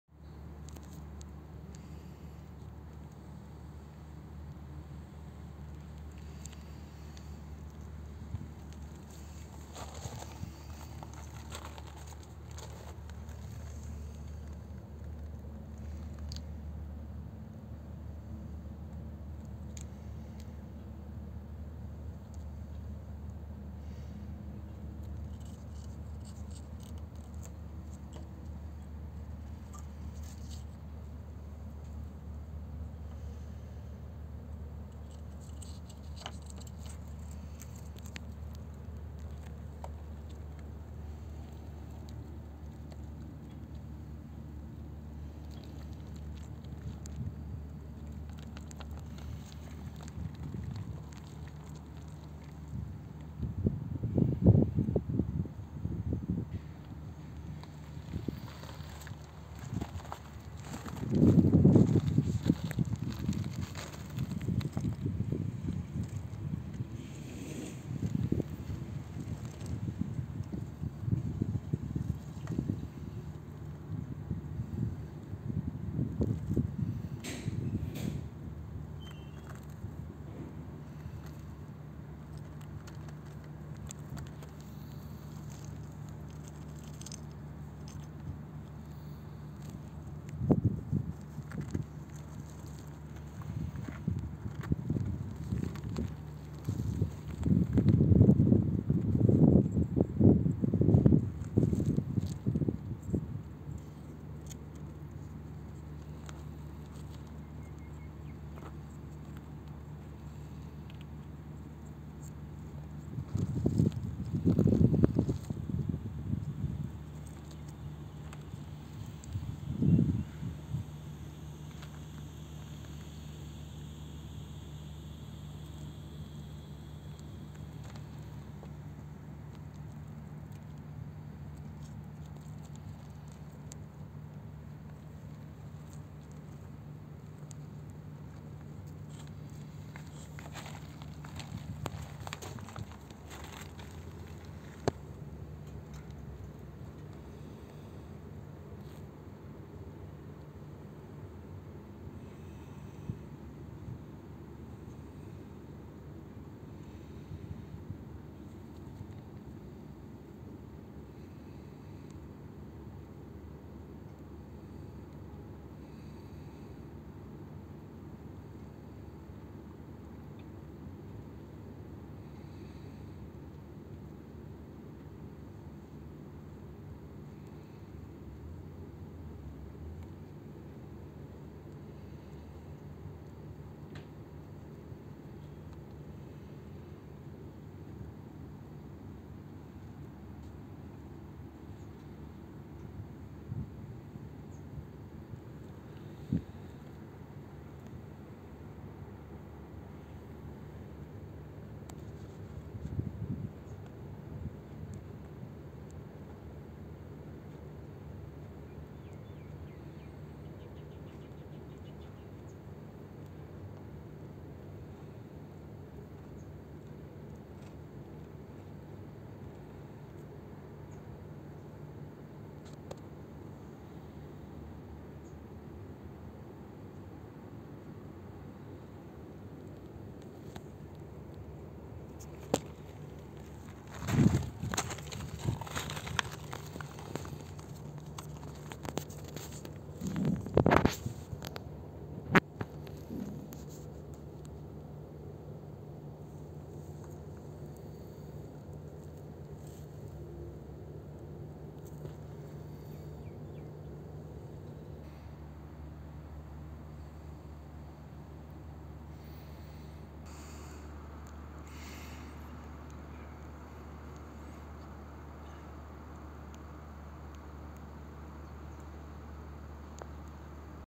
Mole 😄 cute animals 🐒🦥🦧
Different Mole searching for the hole the way to his home, he miss the way. 😂🤣

Cute animals and Cute baby animals, Compilation cutest moment of the animals.